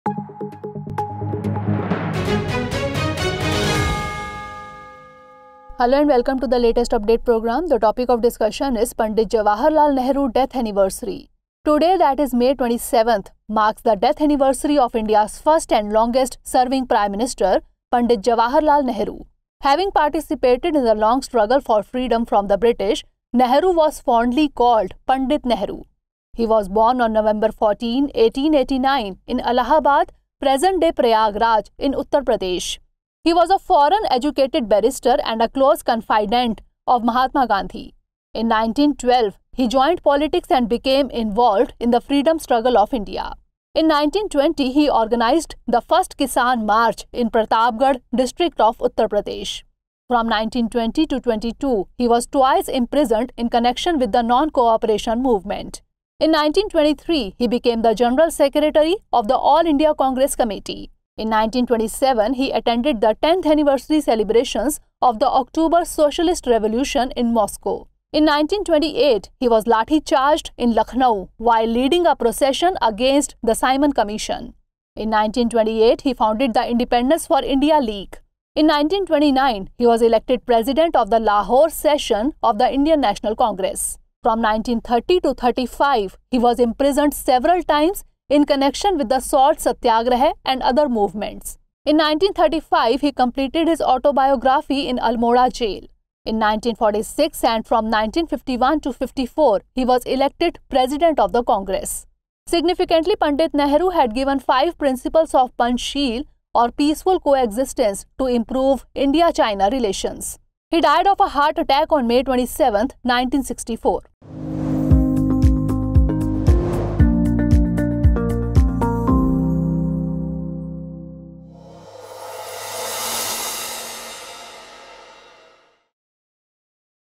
Pt. Jawaharlal Nehru’s Death Anniversary - Latest Update I Drishti IAS English
Pt. Jawaharlal Nehru was born on November 14, 1889, in Uttar Pradesh’s Allahabad (present Prayagraj) and breathed his last on May 27, 1964. He was the independent India’s first and longest serving Prime Minister.

Dear Viewers,

For online order click on these links :